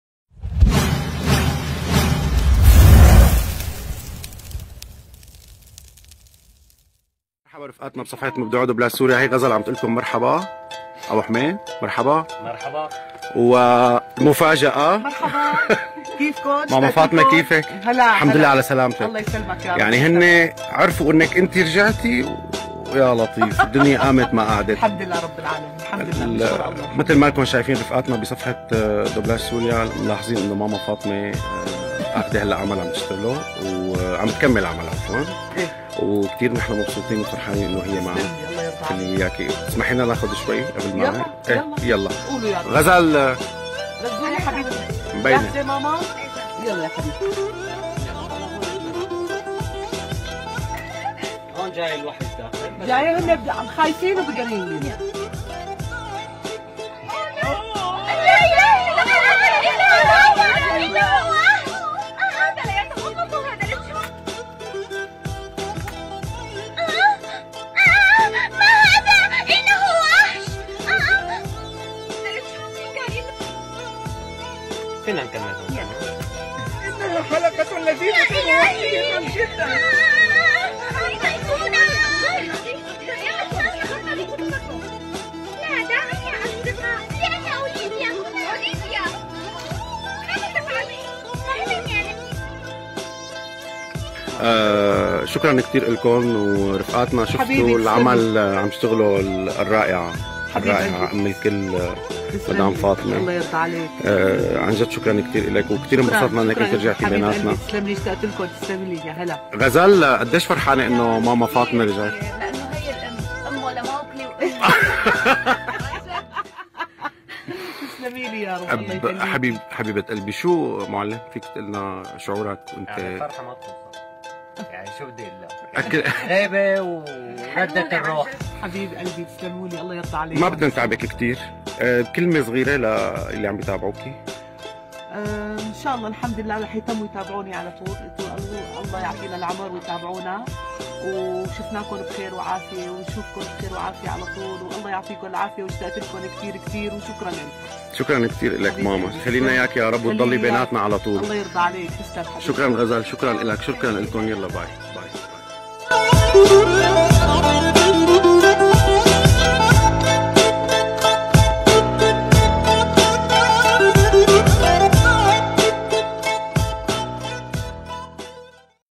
الراااائعة المبدعة " فاطمة سعد " كواليس دوبلاج كرتون
- كواليس دوبلاج مع الفنانة فاطمة سعد والفنانة غزل حنون في شركة اوان او يونايتد سابقا .